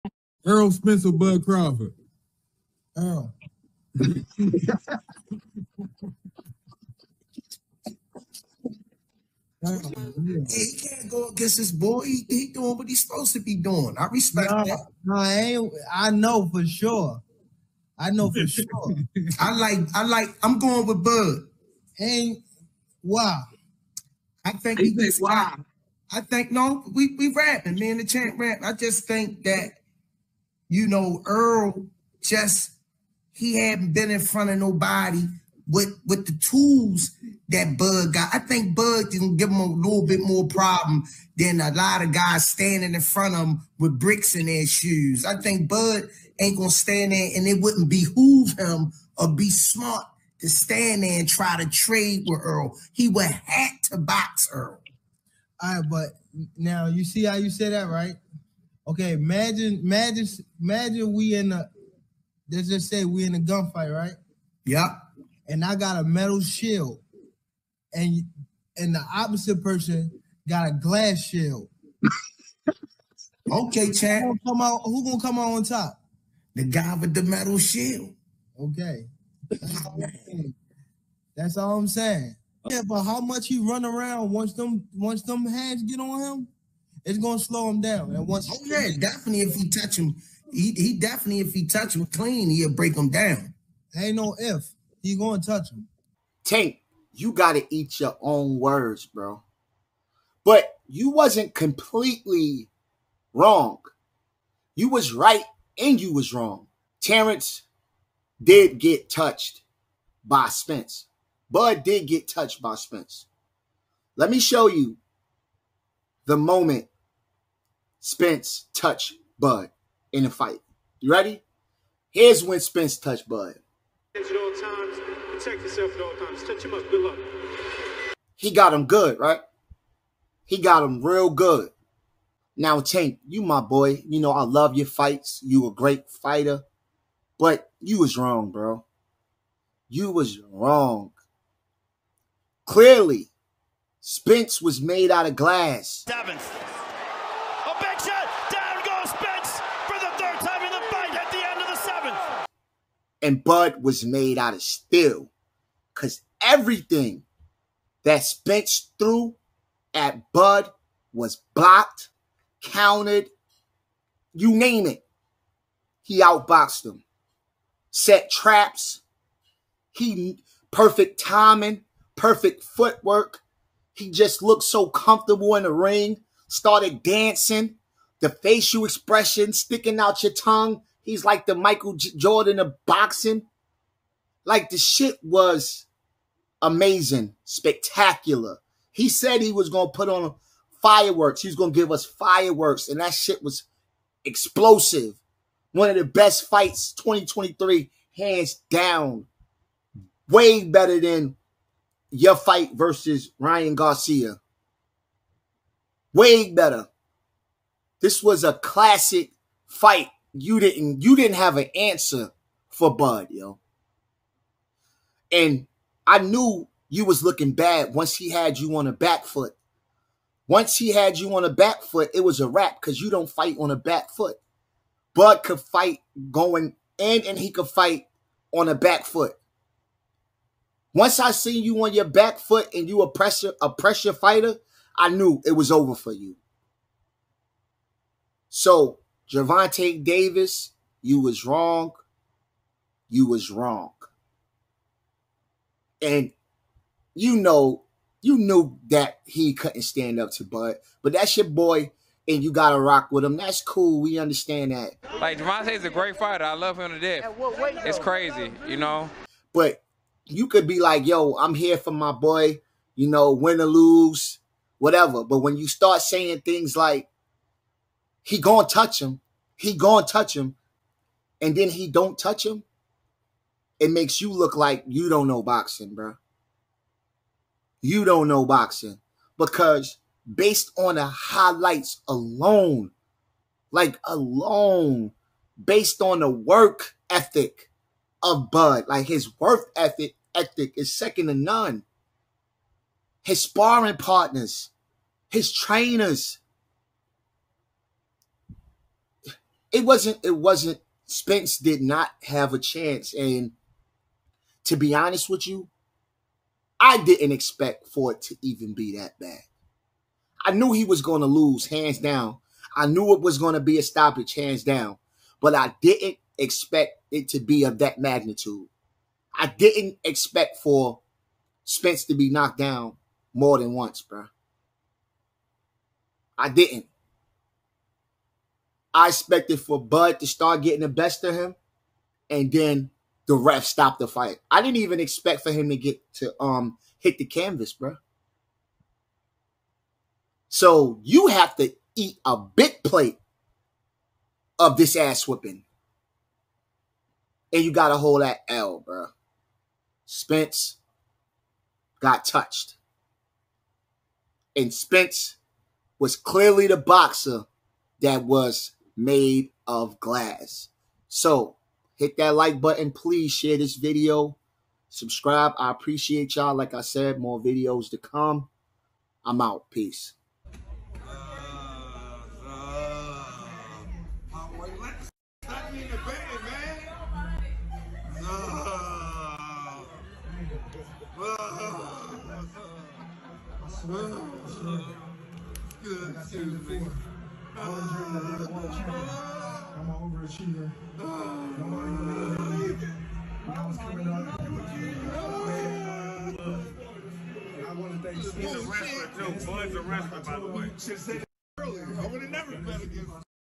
GERVONTA TANK DAVIS HAS TO EAT HIS OWN WORDS BECAUSE ERROL SPENCE HAD A GLASS SHEILD AND NOT BUD!
Gervonta Tank Davis has to eat his own words.